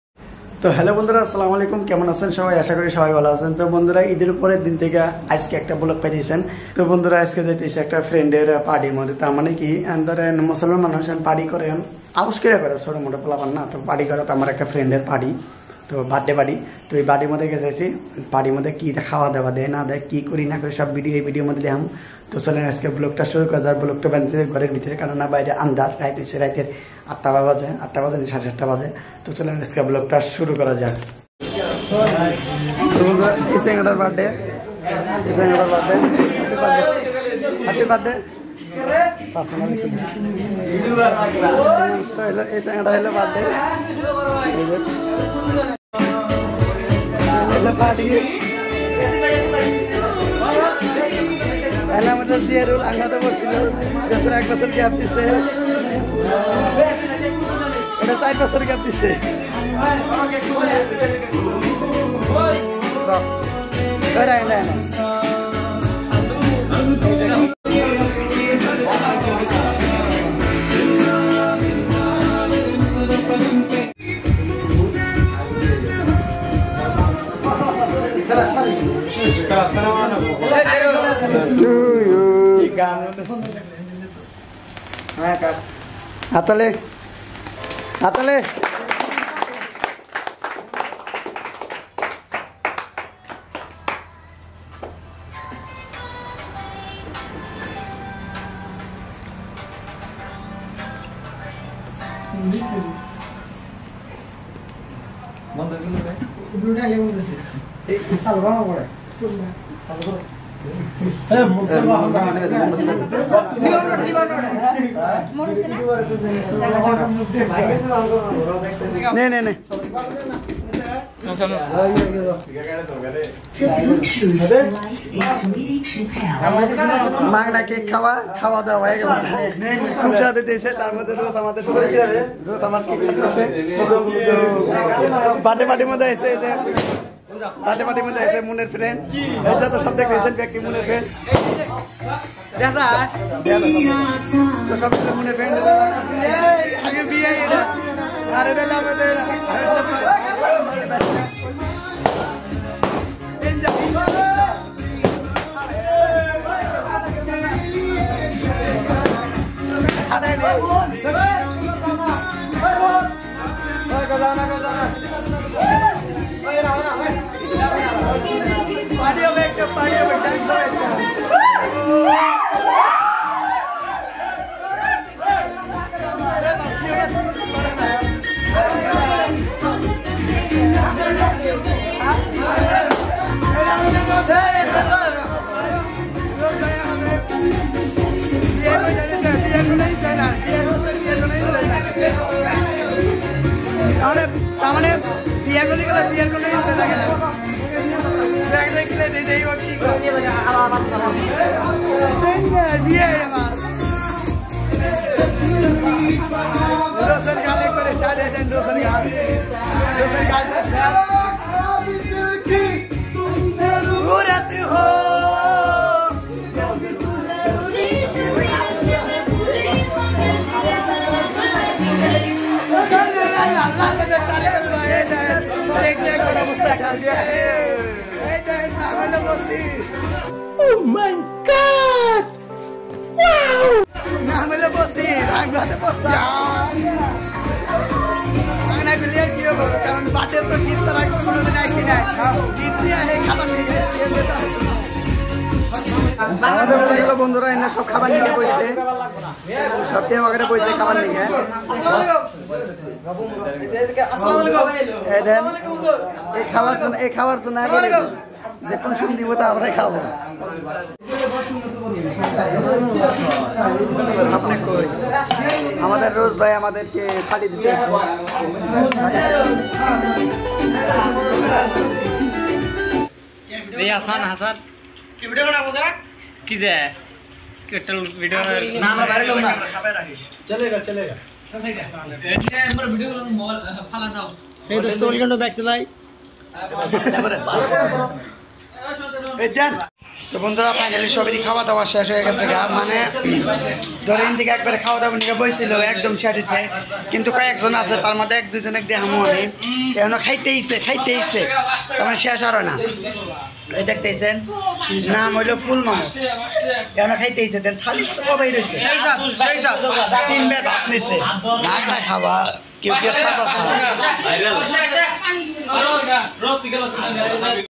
Friend Birthday party 🎉 | বন্ধুৰ জন্মদিন পার্টি | বন্ধুৰ জন্মদিন পাৰ্টিতে কি কৰলাম ?
Friend Birthday party 🎉 | বন্ধুৰ জন্মদিন পার্টি || Bangla Vlogs Video | Eachin Mamud |‎@DimpusVlogs

hello bondhura ai video ami amr frnd barite party giyesilam. tar birthday party giye video baniyesi.